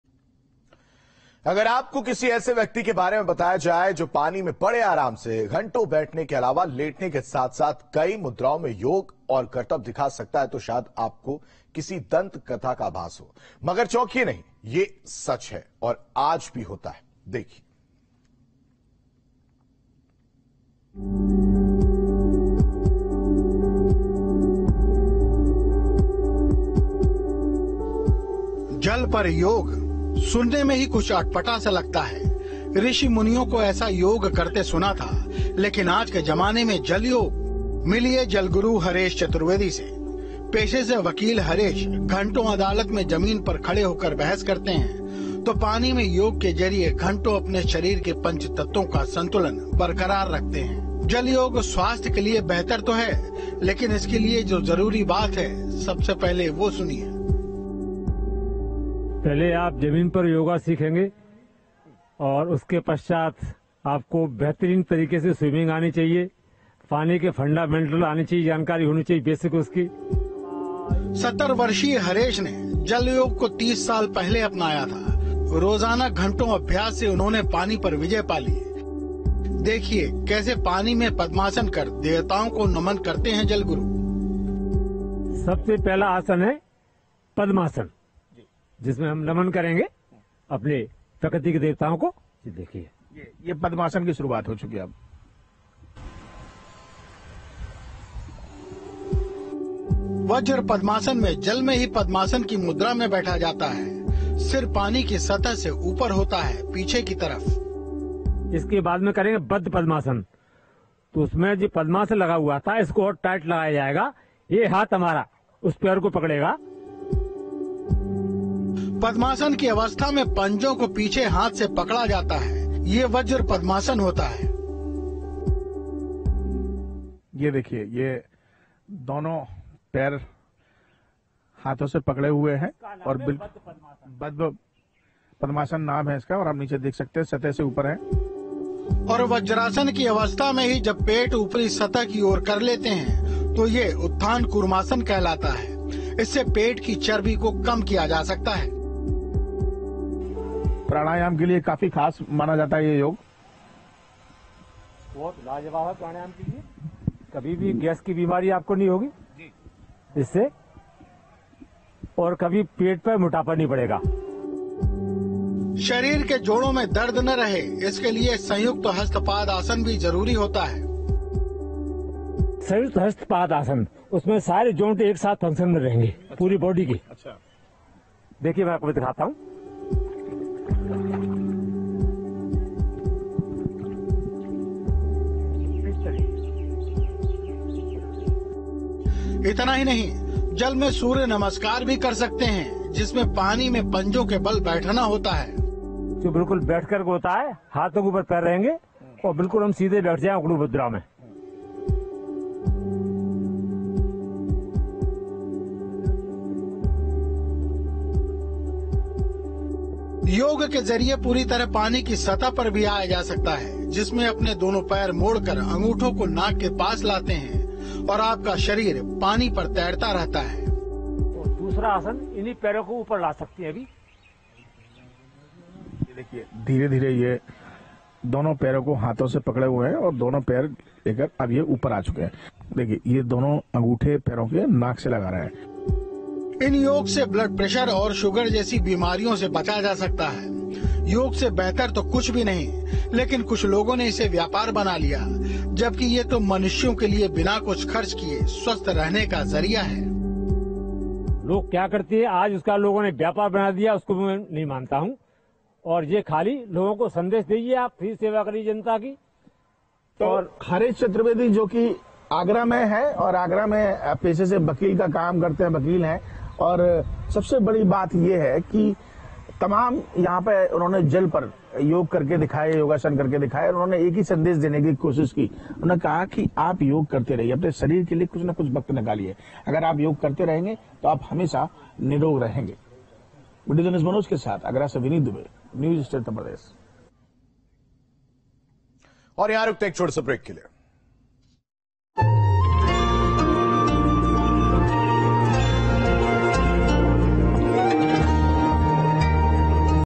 International Yoga day 2022 : Agra के हरेश चतुर्वेदी जमीन पर नहीं जल पर योग करते हैं, देखें Video
जल पर योग.. सुनने में ही कुछ अटपटा सा लगता है.. ऋषि मुनियों को ऐसा योग करते सुना था.. लेकिन आज के जमाने में जय योग.. मिलिए जल गुरु हरेश चौबे से.. पेशे से वकील हरेश घंटों अदालत में जमीन पर खड़े हो कर बहस करते हैं.. तो पानी में योग के जरिए अपने शरीर के पंचतत्वों का संतुलन बरकरार रखते हैं.. जल योग स्वास्थ्य के लिए बेहतर तो है.. लेकिन इसके लिए जो ज़रूरी बात है वो पहले ज़रूर सुनिए

Yoga on water.. sounds a bit strange to hear.. had heard sages doing such yoga.. but in today's era, Jai Yoga.. Meet water guru Haresh Chaubey.. Haresh, a lawyer by profession, landed in court for hours. But let's argue by standing.. So through yoga in water, we maintain the balance of the five elements of our body.. Water yoga is better for health.. But what is important for this, must listen first.

News State Apps डाउनलोड करें:
Download Android and iOS Apps for more updates: